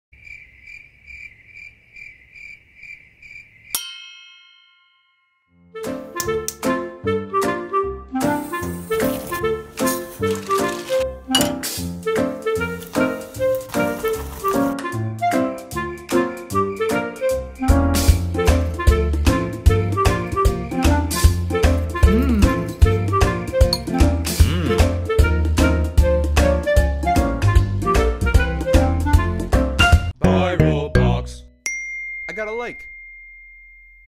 WINNING AD - MARCH CONTEST - by You+Me Creative
WINNING AD for Virlbox's March Contest submitted by You+Me Creative (Lisa Krawchuck and Ryan Fitzgibbon)
Want to make Ads too? Sign up at virlbox.com!